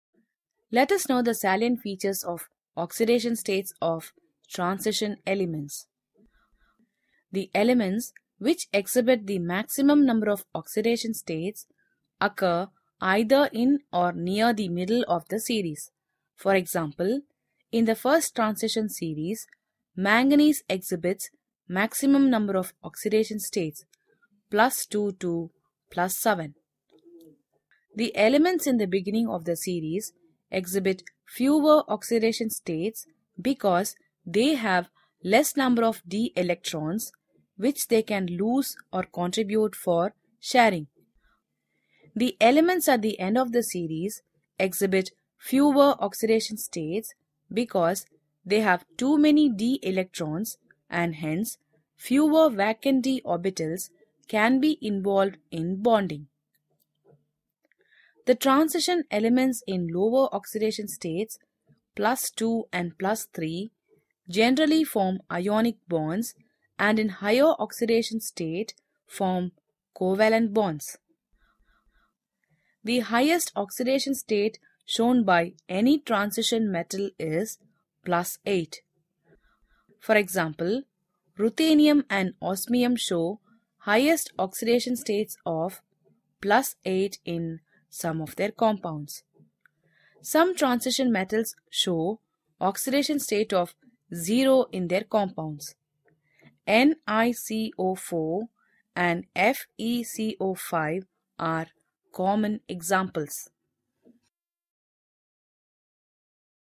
What are the salient features of oxidation states of transition elements ?....Let us Know....
Here is given the main salient features of oxidation states of transition elements.